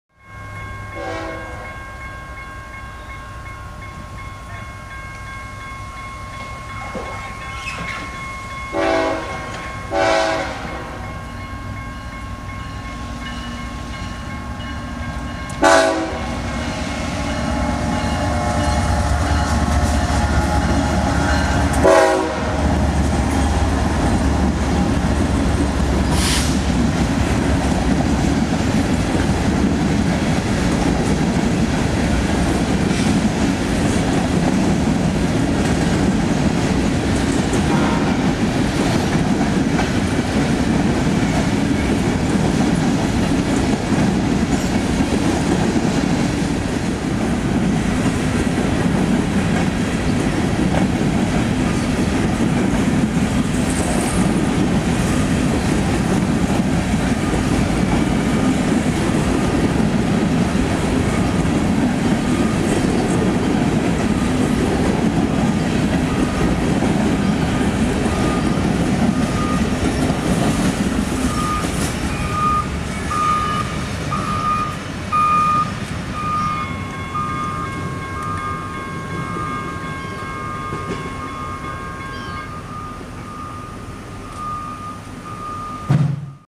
Westbound BNSF Unit Grain Train at Fairview Station in Downers Grove, IL
I filmed this train at Fairview in Downers Grove as part of a log of trains that passed while I watched them replace the track for M3 in the street crossing. After I saw it was a unit train, I quit because they get boring fast. But then when I noticed the train was picking up speed, I made the segment starting at 0:47.